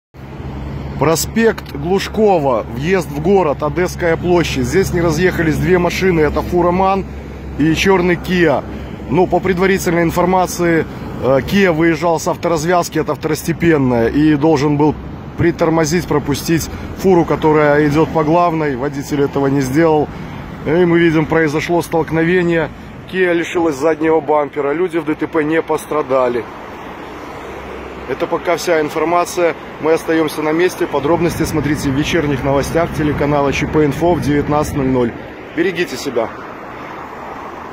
ДТП на Одеській площі
ДТП на Одеській площі. Нерозїхались фура ДАФ та КІА. Люди в ДТП не постраждали.  КІА виїжджала з авторозв'язки та не пропустила фуру яка їхала по головній...